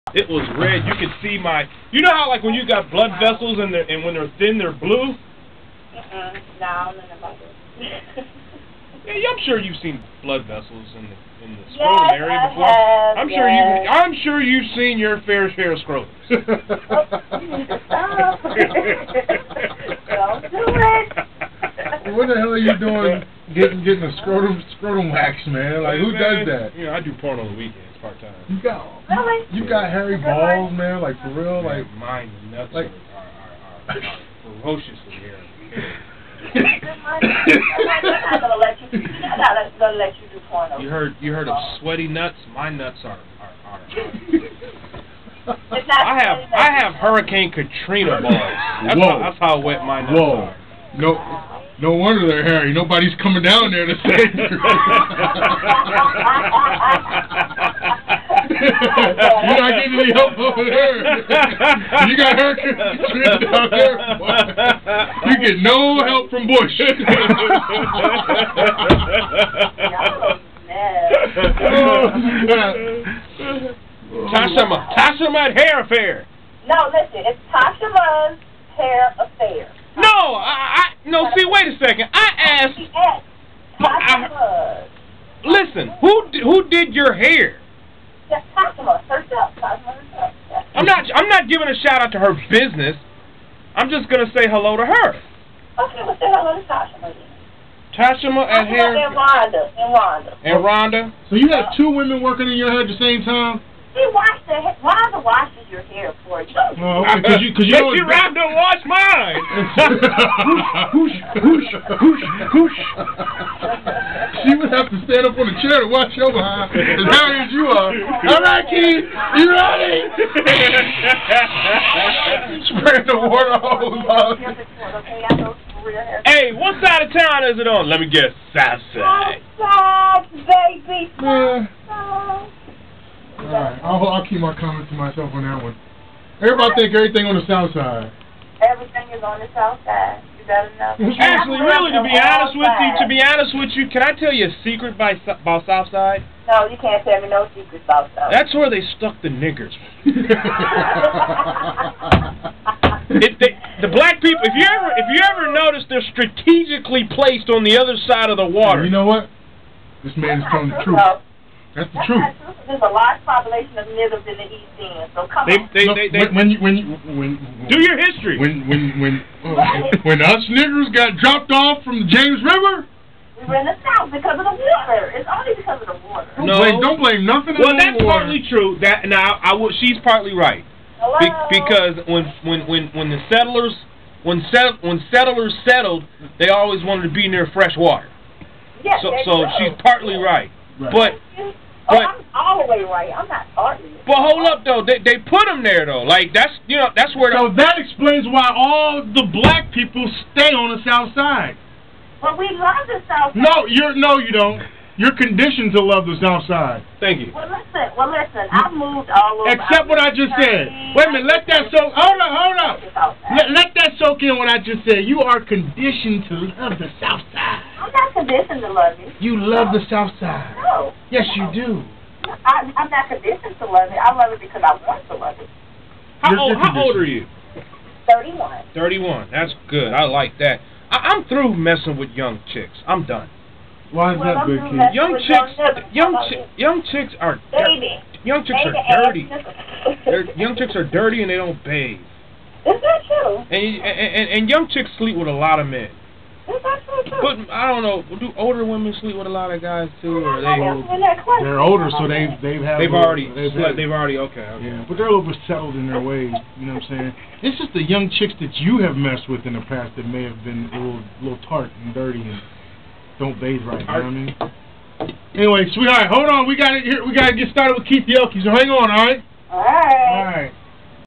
(M.N.E.D.) RAW REAL & UNCUT RADIO W/ BIG KEITH & SEAN ANTHONY
SEAN & KEITH TALK TO A LISTENER ABOUT SOME RAW STUFF. LISTEN